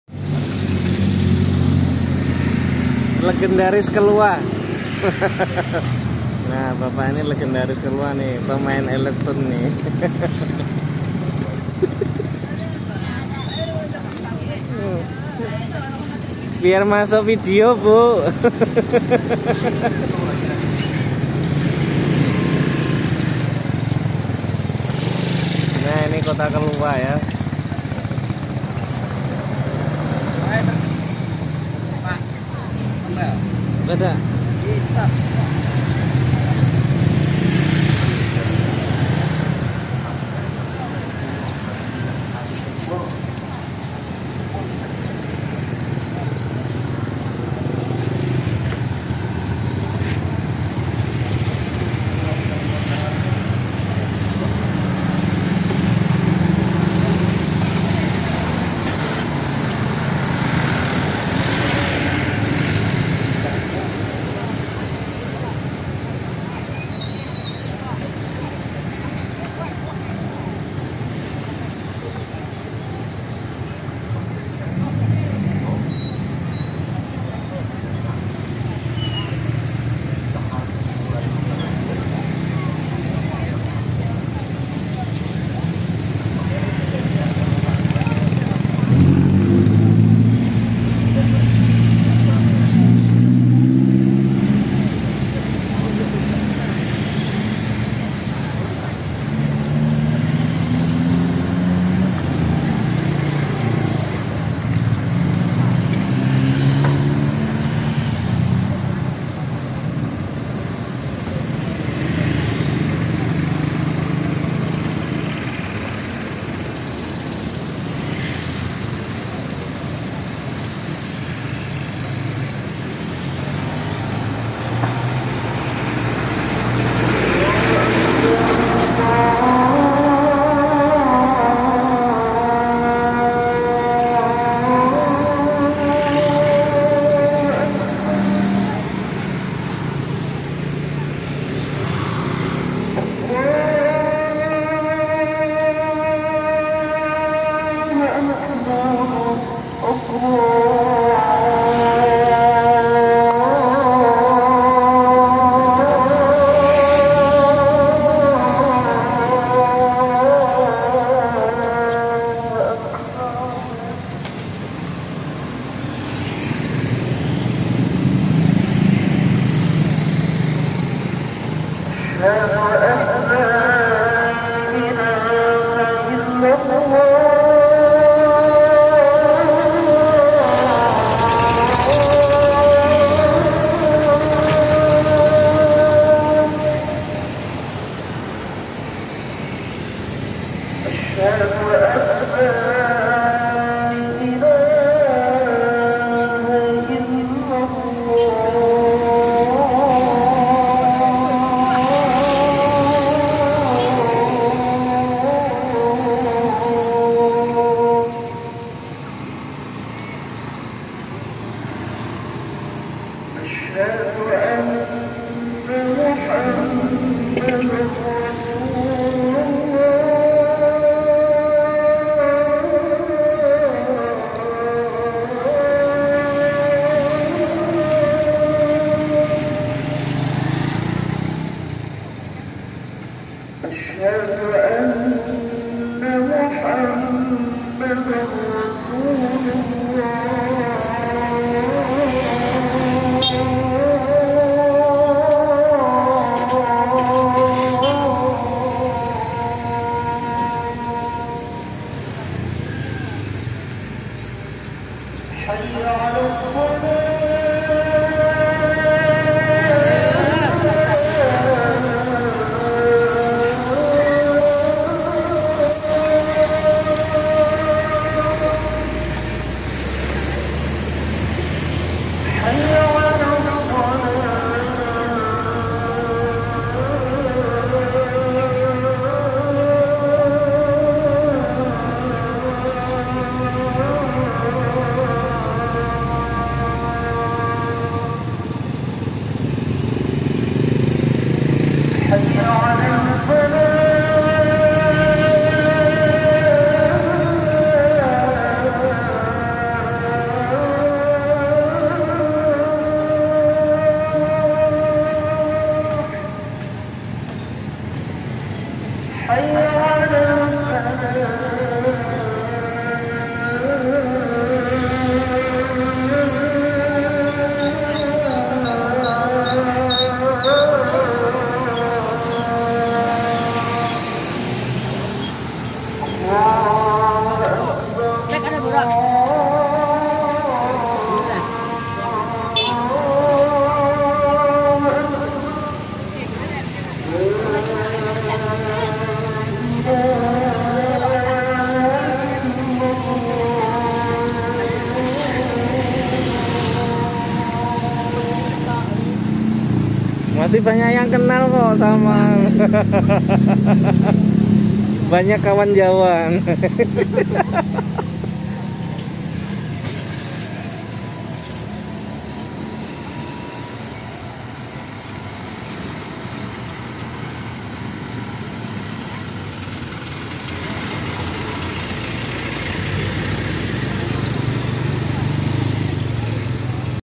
6 menit spotting di depan pasar kalua sore menjelang maghrib|malam pasaran hari kamis di kota kelua
spotting suasana sore hari menjelang maghrib pas kebetulan hari malam pasaran kota kelua, pasar terbesar wilayah selatan kota tabalong